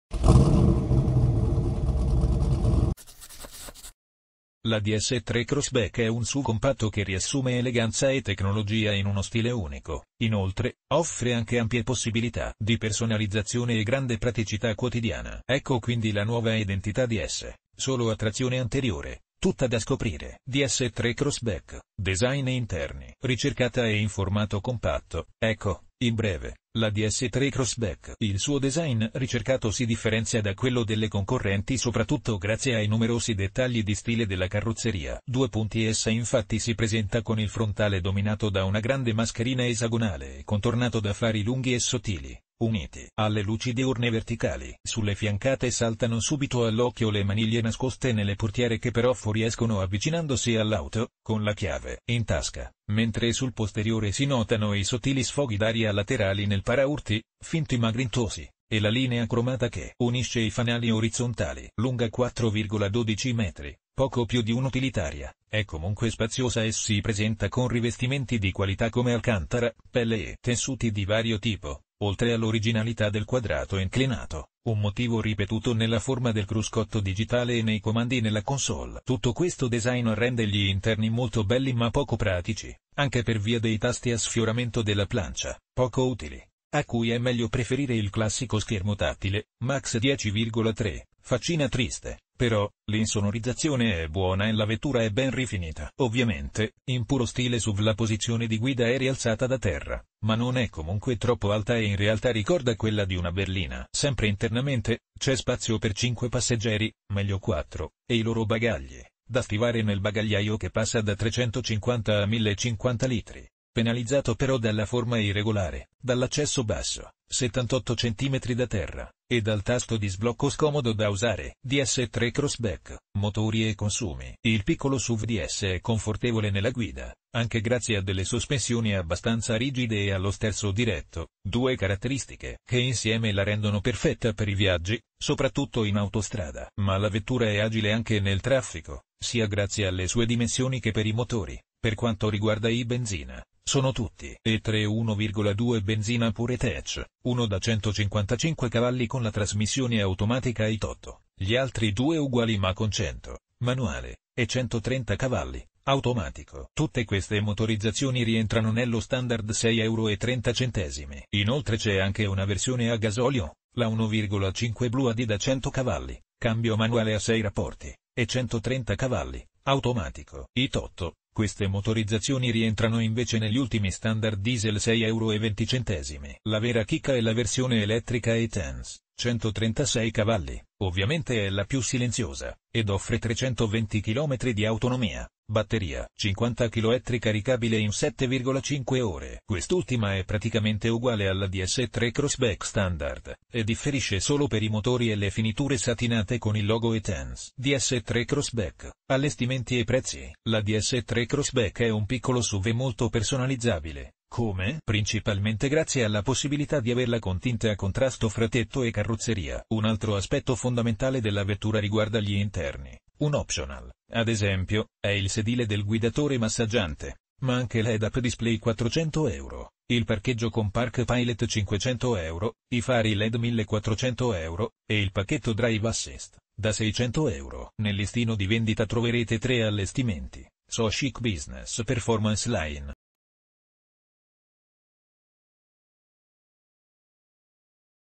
DS 3 Crossback: un vero gioiello su ruote - scopri prezzo e opzioni d'allestimento!
Accompagnaci nel mondo del DS 3 Crossback, un'auto che unisce design d'avanguardia e funzionalità d'eccellenza. In questo video, ti guideremo attraverso le diverse opzioni d'allestimento disponibili e, naturalmente, ti sveleremo il prezzo di questo capolavoro automobilistico. Se hai l'occhio per il dettaglio e la passione per l'eleganza, non vorrai perderti questo tour!